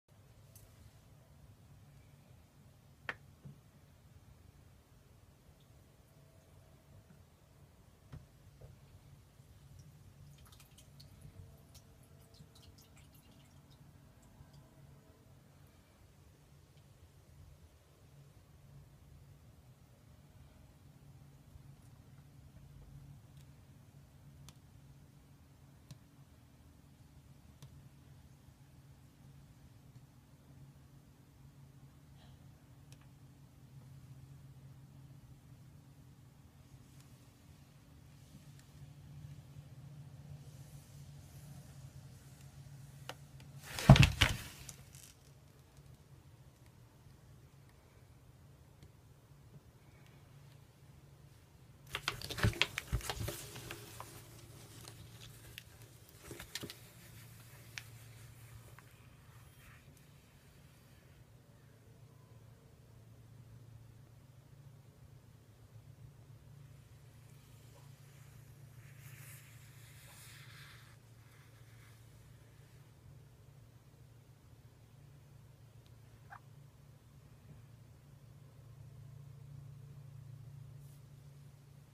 California King Snake Choke Hold
Quick clip of our Cali King Rodney showing off his wrapping skills.